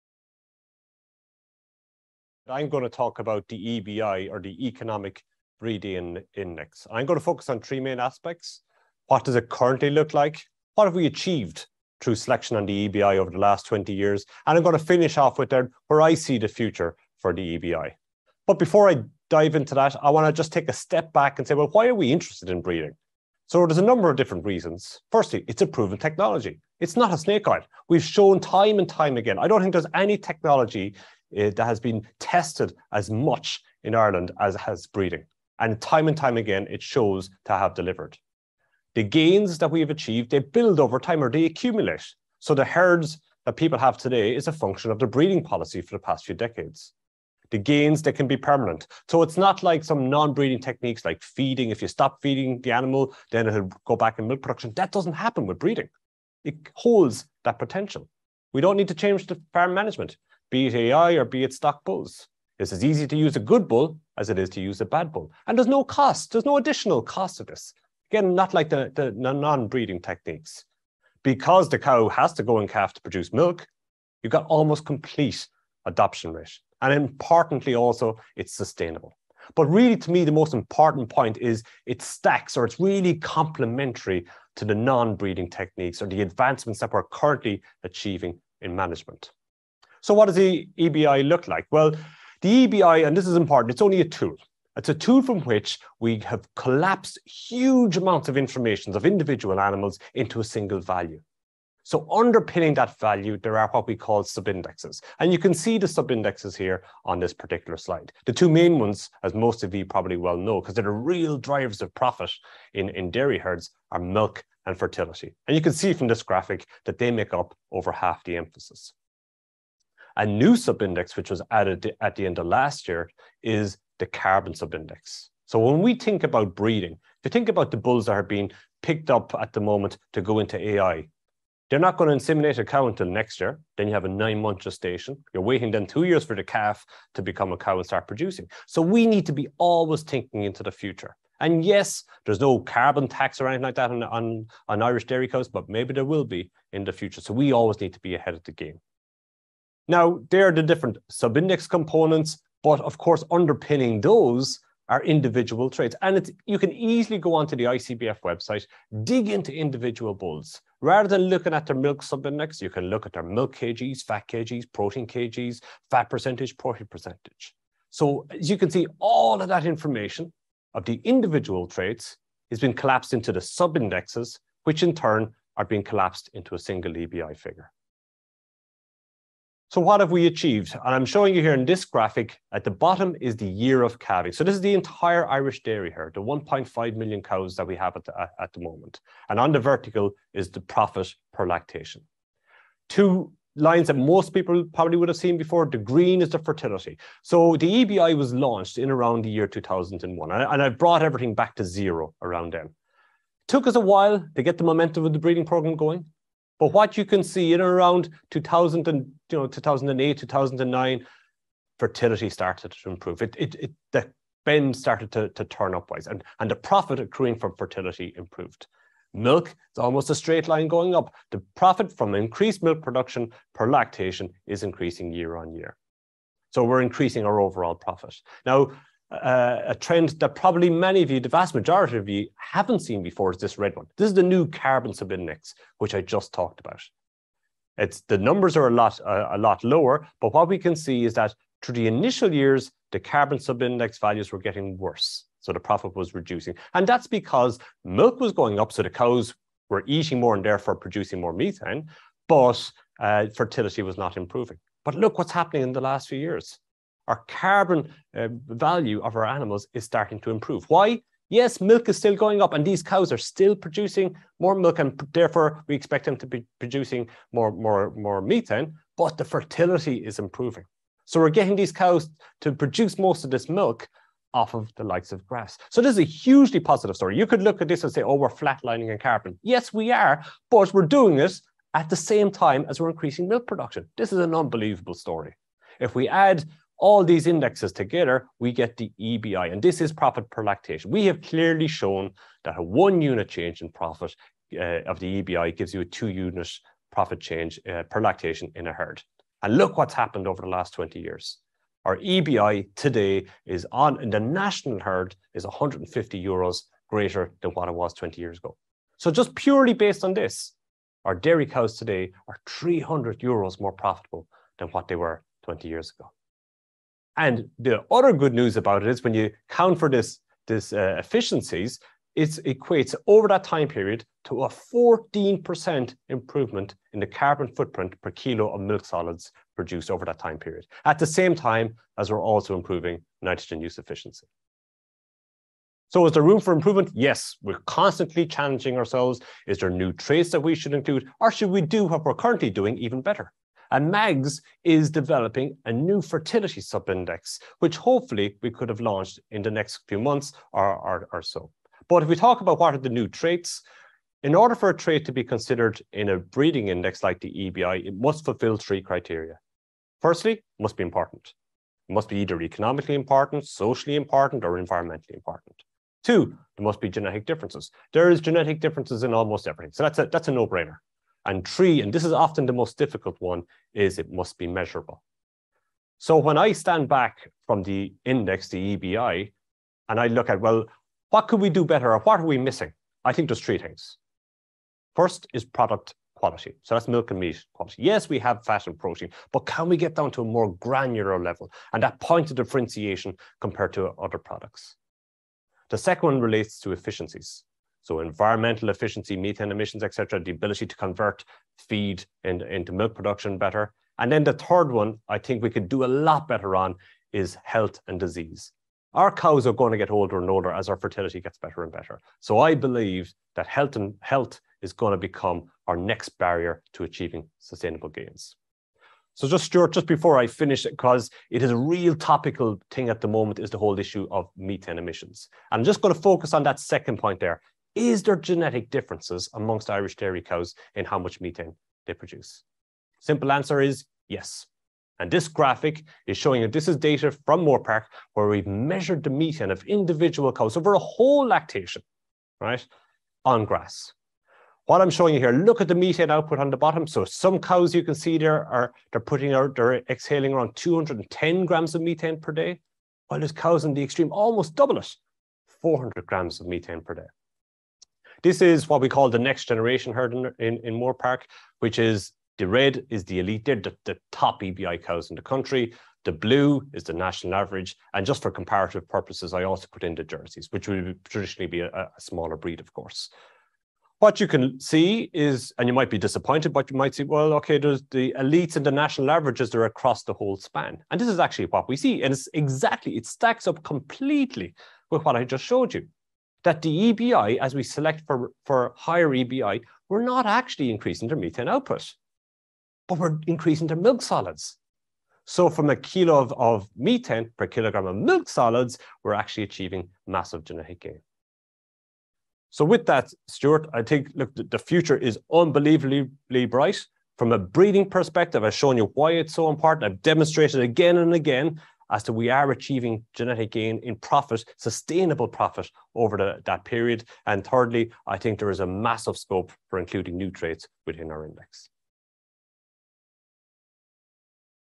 THE EBI - An Economic Breeding Index for the future - Donagh Berry
Breeding boasts the advantages of being a proven technology that has clearly been demonstrated over decades to be able to deliver changes in performance. The benefits from breeding cumulate over time and can be permanent – while an advantage, this is can also be a disadvantage in that a lapse in concentration or interest in breeding leading to the wrong choice of bulls for any given year can cause repercussions which could take years to purge out of a herd. Because cows have to produce a calf to milk, the technology of breeding has a 100% adoption rate – additionally, it requires no change in farm management as the cow has to establish pregnancy so it is as easy to use a good bull as use a bad bull; because the cost differential between a good bull and bad bull is often very low, good breeding practices do not incur an additional cost. Moreover, the benefits of breeding are complementary with non-breeding techniques with the benefits of both stacking to further accelerate the rate of change in performance.

The Irish national breeding index, the Economic Breeding Index (EBI), is comprised of eight sub-indexes related to milk production, fertility & survival, calving performance, beef performance, carbon output, cow maintenance (i.e., size), health, and management. The impact of each sub-index on profit is simply summed together to generate a single expected profit figure per animal – this figure is the EBI.  Each sub-index has a series of underpinning traits each weighted by their calculated effect on farm profit. While EBI is the unit of currency of individual bulls and cows, it is advised to also examine the sub-indexes of the candidate parents of the next generation and choose those most suited to the farming system where they will be used. Given that breeding contributes half the gains in performance observed across the world, irrespective of species, it is crucial that effort and time is put into properly selecting the best sires and dams for future generations.